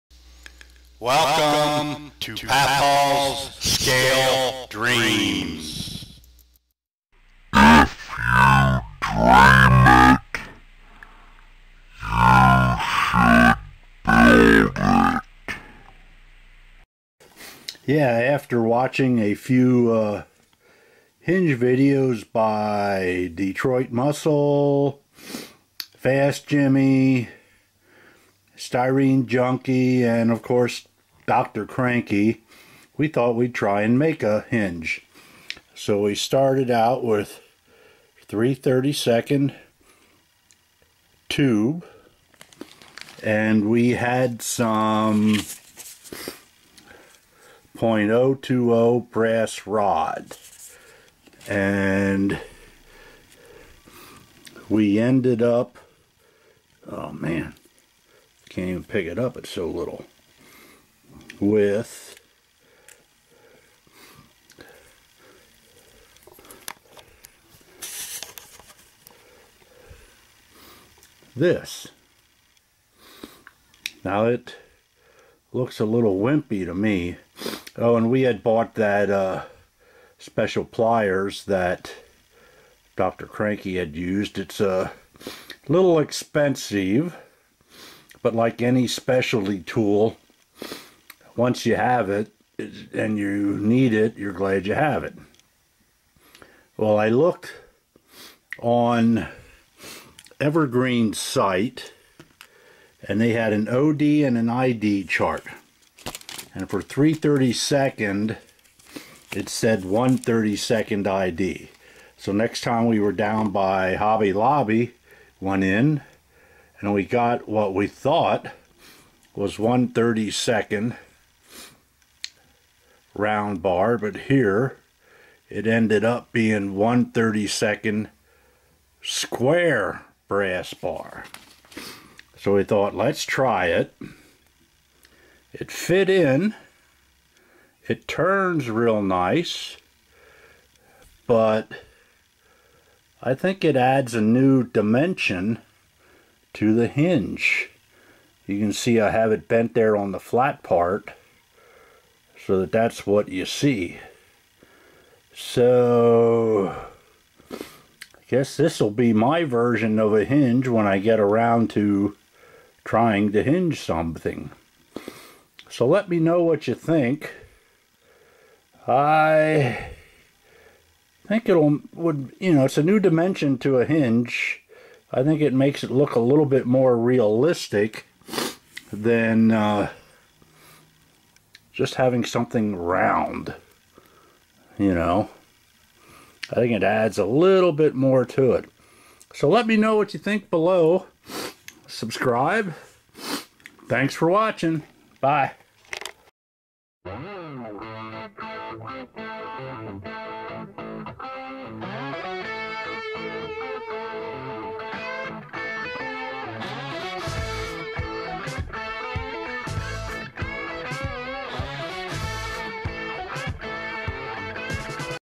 Let's Make A Hinge
Pappaw's Scale Dreams
If you dream it
You should build it
I tried to make a hinge and ended up with this!!
Pappaw's Hinge